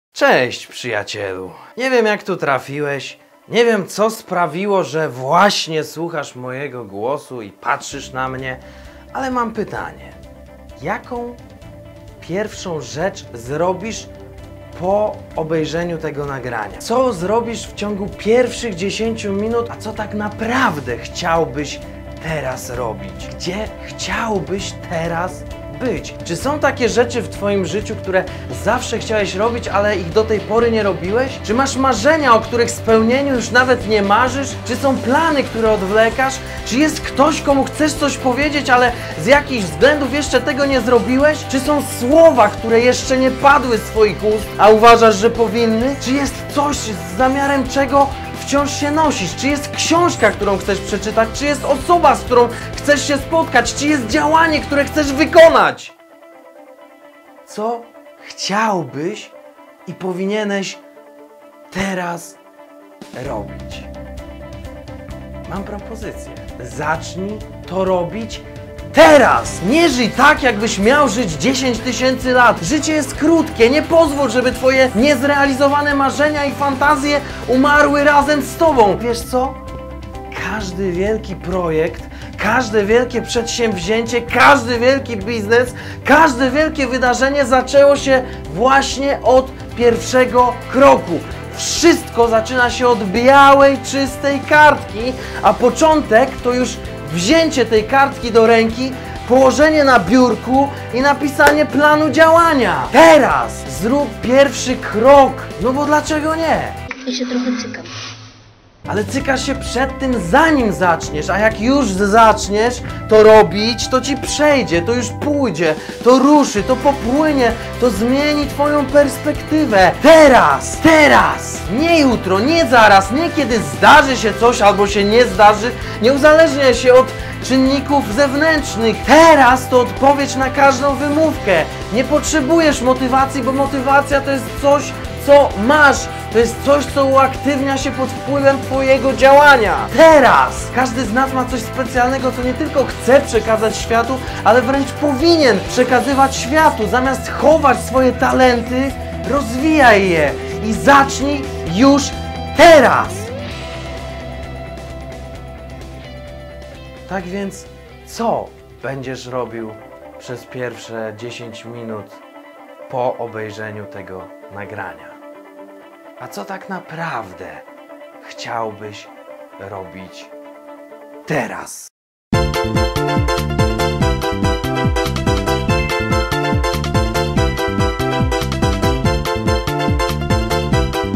MOTYWACJA W 3 MINUTY!
Czasami potrzebujemy kogoś kto nam powie ,,ZRÓB TO TERAZ!". Czasami wystarczy jeden mały bodziec aby podjąć nowe działanie. Niech to nagranie będzie dla Ciebie właśnie takim impulsem!
TERAZ!

Dzięki, że jesteś! :)